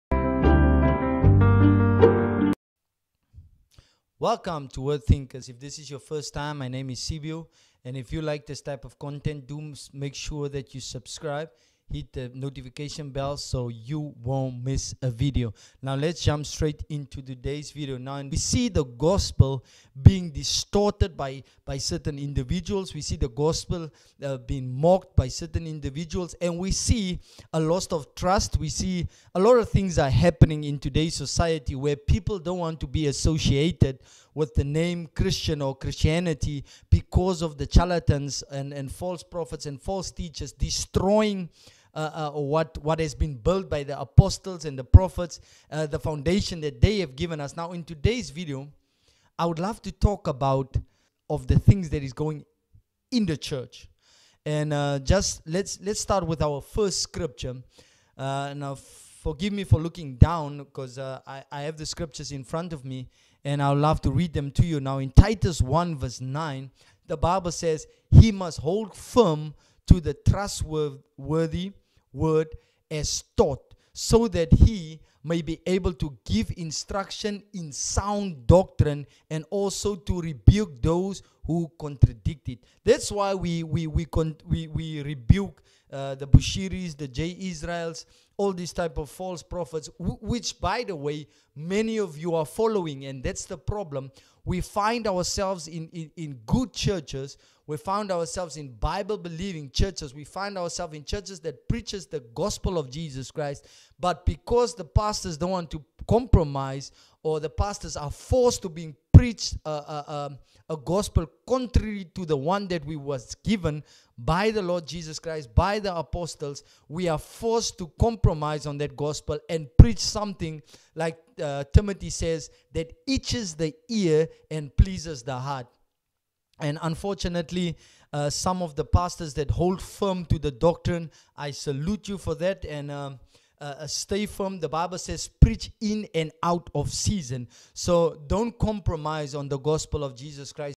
Jay israel rebuke and all the false prophets | different gospel.
Jay israel bushiri alpha lukau are the visible false prophets seen but not avoid by so many the Bible says mark them and avoid them reject them but are we doing that as Christians?
false prophets exposed seer 1, fake miracles in the church and fake pastors lying and deceiving many the Lords words beware of false prophets Matt7:13.

Sound doctrine is essential to our Christian faith and in today's society there are many defectors then defenders of the faith. Jude call us to Contend for the faith.

But when we send them money what are we doing? Increase their influence and contributing to the damage. video to this topic

Bible verse
Titus 1:9

Lets contend for the faith let's fight for the faith.